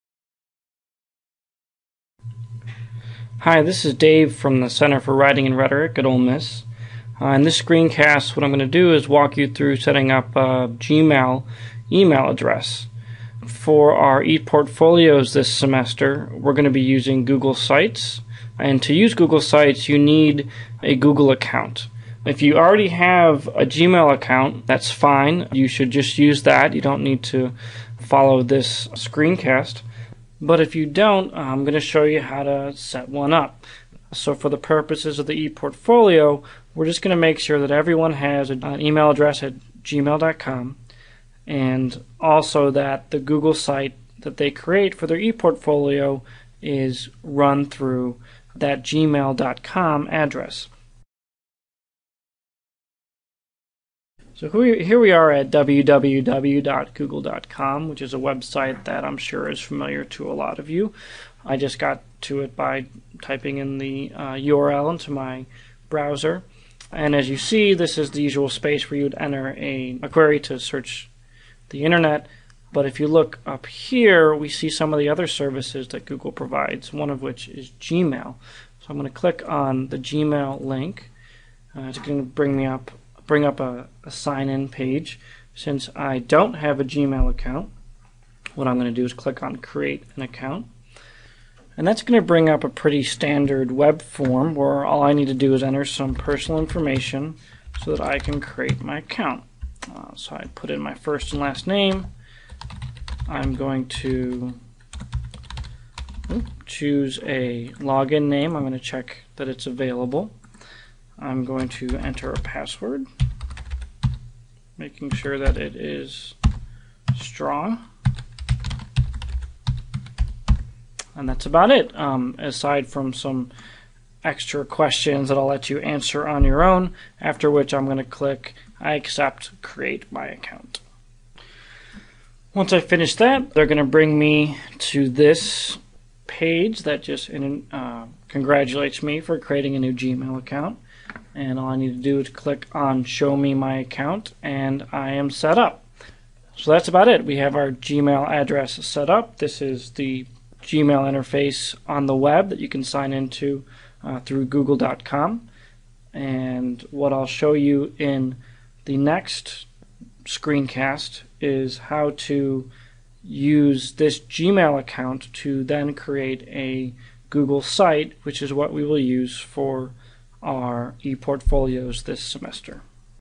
CWR: Setting up a Gmail Account
In this screencast, we walk you through the process of setting up a Gmail account, which you will use to create a Google Site for your ePortfolio